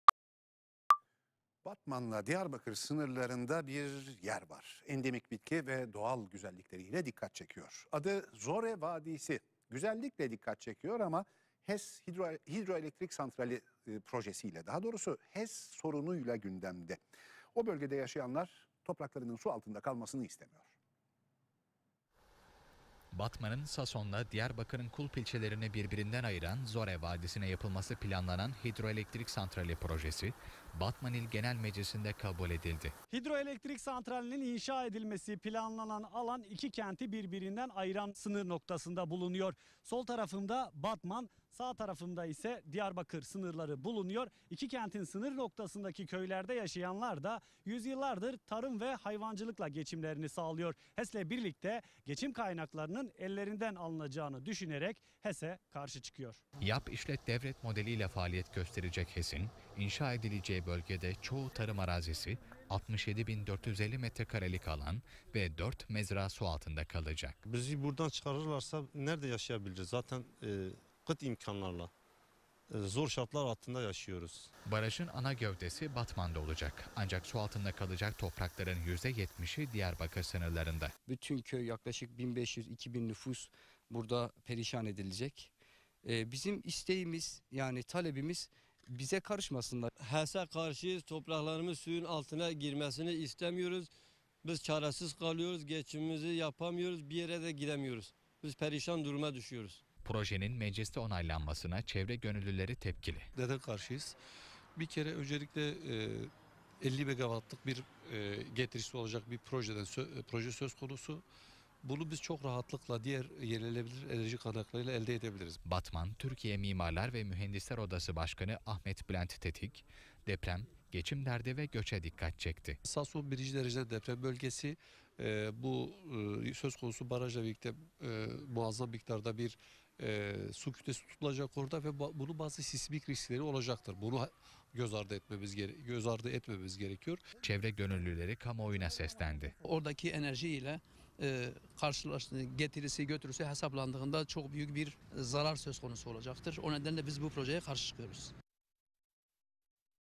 Zore Vadisi'nde HES tepkisi
Diyarbakır ile Batman'ın tam ortasında yer alan Zore Vadisi'nde HES yapılması kararı, bölgede yaşayanların tepkisine neden oldu